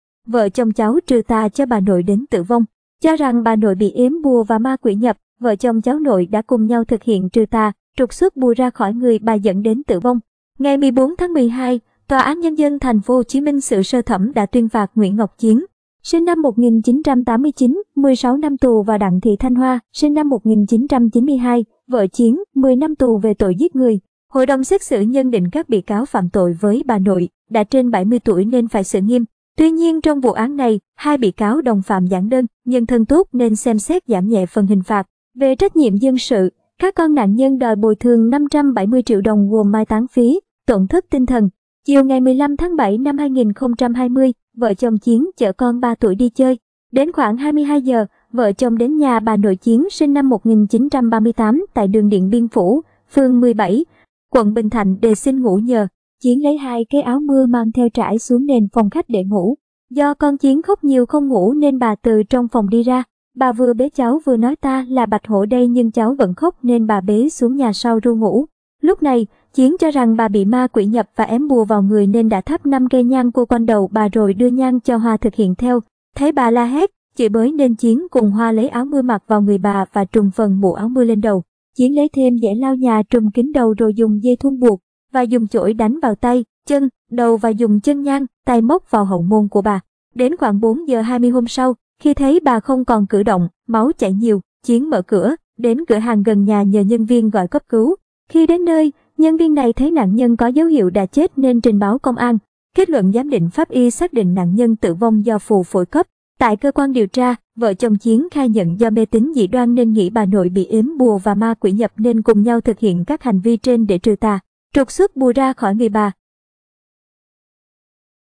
Vợ chồng cháu 'trừ tà' cho bà nội đến tử vong
Cho rằng bà nội bị ếm bùa và ma quỷ nhập, vợ chồng cháu nội đã cùng nhau thực hiện 'trừ tà, trục xuất bùa' ra khỏi người bà dẫn đến tử vong.Ngày 14-12, TAND TP.HCM xử sơ thẩm đã tuyên phạt Nguyễn Ngọc Chiến (sinh năm 1989) 16 năm tù và Đặng Thị Thanh Hoa (sinh năm 1992, vợ Chiến) 10 năm tù về tội giết người.
Kết luận giám định pháp y xác định nạn nhân tử vong do phù phổi cấp.
Về trách nhiệm dân sự, các con nạn nhân đòi bồi thường 570 triệu đồng gồm mai táng phí, tổn thất tinh thần...
Chiều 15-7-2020, vợ chồng Chiến chở con ba tuổi đi chơi. Đến khoảng 22 giờ, vợ chồng đến nhà bà nội Chiến (sinh năm 1938) tại đường Điện Biên Phủ, phường 17, quận Bình Thạnh đề xin ngủ nhờ.
Chiến quay lại nói chuyện với vợ (được tại ngoại) khi tòa nghị án. Ảnh: H.YẾN
Lúc này, Chiến cho rằng bà bị ma quỷ nhập và ém bùa vào người nên đã thắp 5 cây nhang quơ quanh đầu bà rồi đưa nhang cho Hoa thực hiện theo.
HĐXX nhân định các bị cáo phạm tội với bà nội, đã trên 70 tuổi nên phải xử nghiêm. Tuy nhiên trong vụ án này, hai bị cáo đồng phạm giản đơn, nhân thân tốt nên xem xét giảm nhẹ phần hình phạt.
Tại CQĐT, vợ chồng Chiến khai nhận do mê tín dị đoan nên nghĩ bà nội bị ếm bùa và ma quỷ nhập nên cùng nhau thực hiện các hành vi trên để trừ tà, trục xuất bùa ra khỏi người bà.
Thấy bà la hét, chửi bới nên Chiến cùng Hoa lấy áo mưa mặc vào người bà và trùm phần mũ áo mưa lên đầu. Chiến lấy thêm giẻ lau nhà trùm kín đầu rồi dùng dây thun buộc. Và dùng chổi đánh vào tay, chân, đầu và dùng chân nhang, tay móc vào hậu môn của bà.
Chiến lấy 2 cái áo mưa mang theo trải xuống nền phòng khách để ngủ. Do con Chiến khóc nhiều không ngủ nên bà từ trong phòng đi ra. Bà vừa bế cháu vừa nói “ta là bạch hổ đây” nhưng cháu vẫn khóc nên bà bế xuống nhà sau ru ngủ.
Đến khoảng 4 giờ 20 hôm sau, khi thấy bà không còn cử động, máu chảy nhiều, Chiến mở cửa, đến cửa hàng gần nhà nhờ nhân viên gọi cấp cứu. Khi đến nơi, nhân viên này thấy nạn nhân có dấu hiệu đã chết nên trình báo công an.
Hai vợ chồng bị cáo Chiến - Hoa. Ảnh: H.YẾN